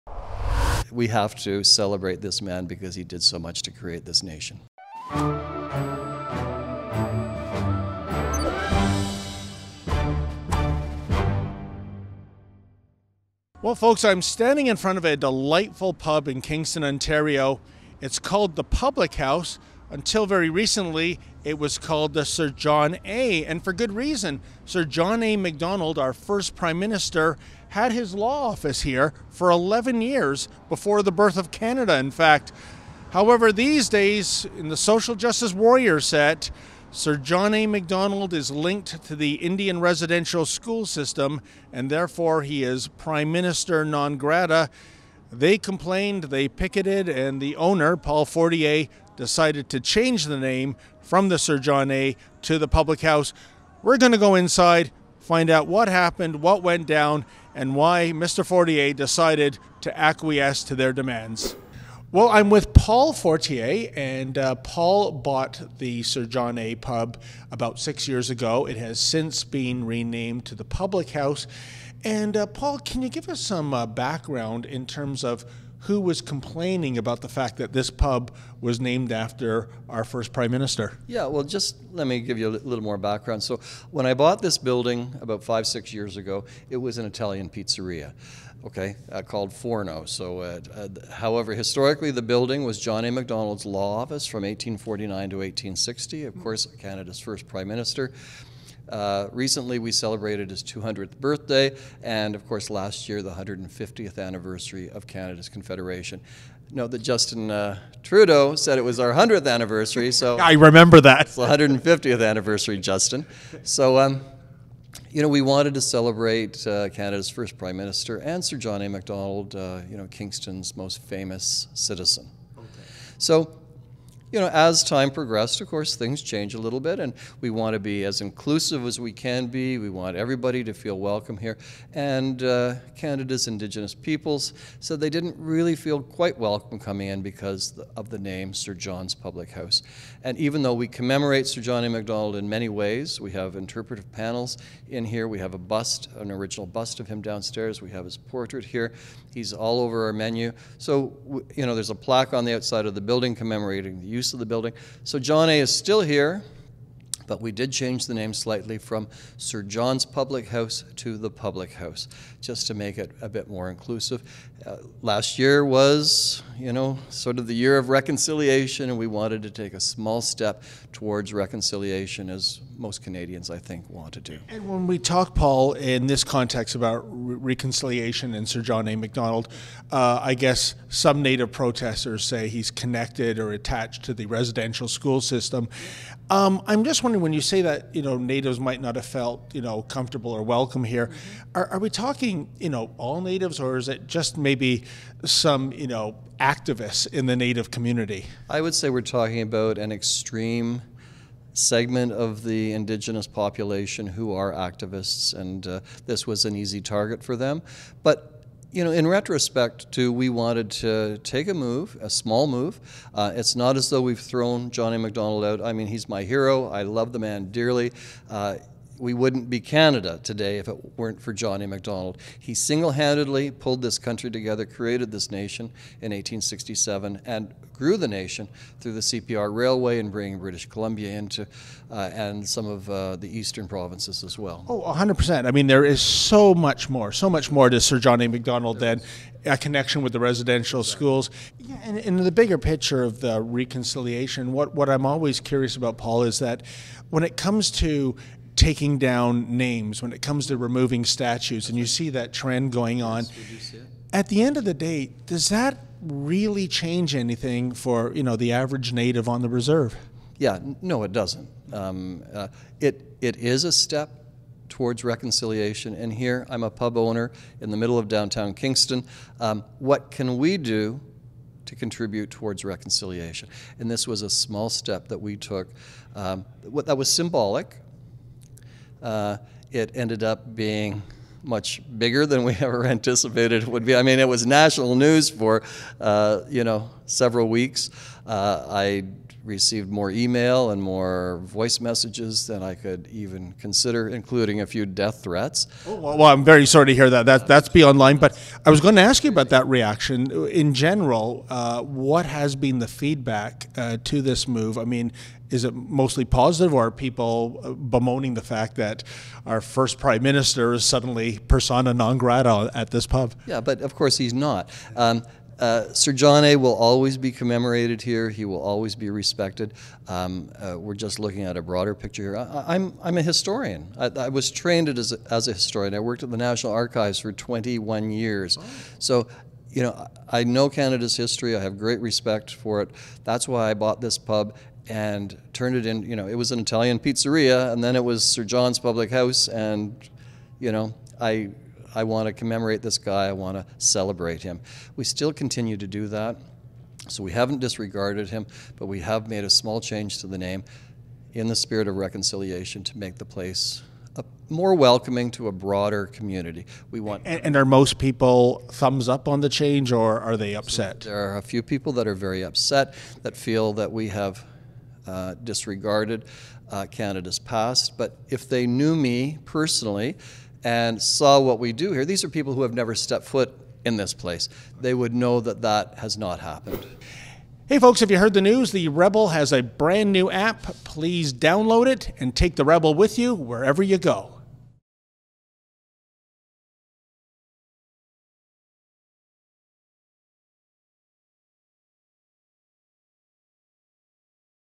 “Death threats” or “reconciliation”? Sir John A. pub owner explains name change
David Menzies of The Rebel.Media reports: Sir John’s Public House in Kingston, Ontario, is now known as The Public House as a result of pressure from a vocal minority to remove our first PM's name. Watch as pub owner Paul Fortier provides some background and explains why he acquiesced in the end. MORE
PS: WATCH David Menzies’ “What’s the Deal” (PLUS shows hosted by Ezra Levant, John Cardillo and MORE)